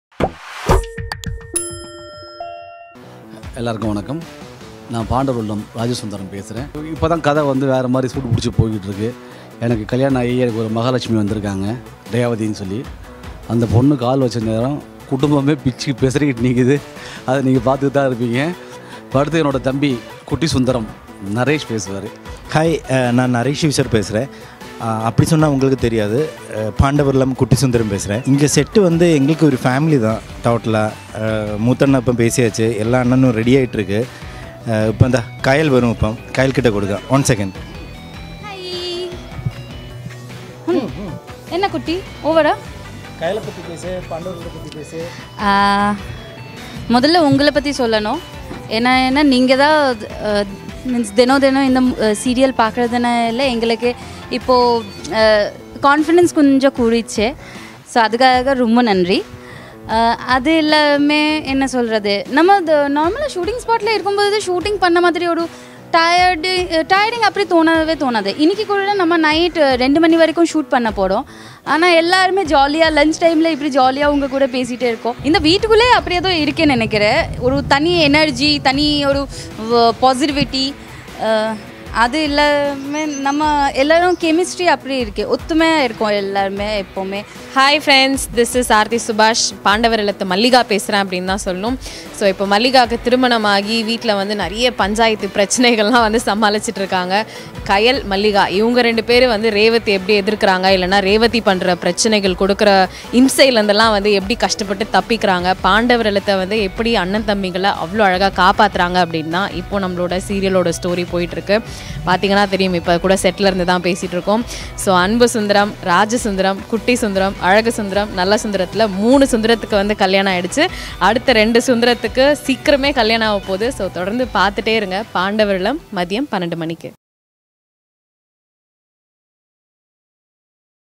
Exclusive interview With Pandavar Illam | Pandavar Illam Special | Sun Digital Exclusive
The team of Pandavar Illam take you behind the scenes of the serial set